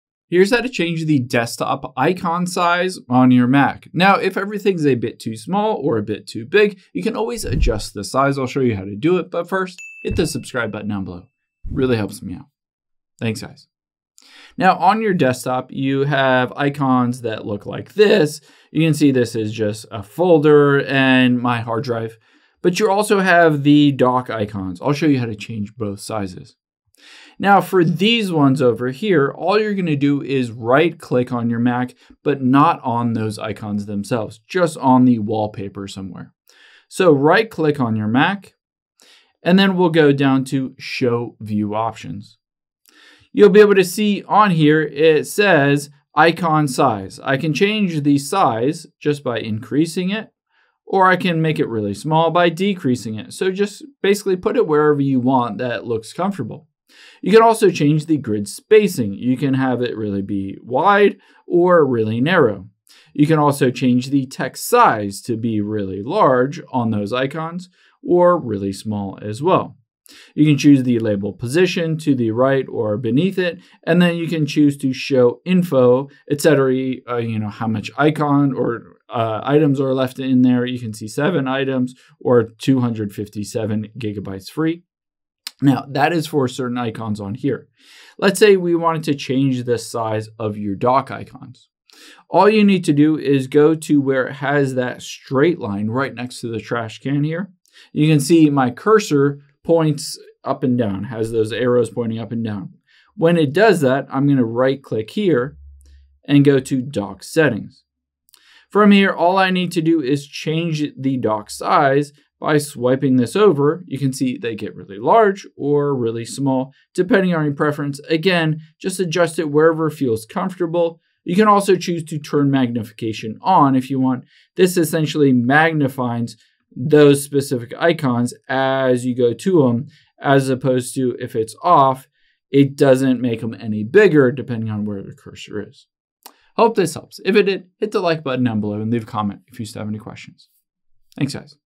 How To Change Desktop Icon Size On Mac
Let's change the size of any desktop icons if you want them bigger or smaller on your Mac.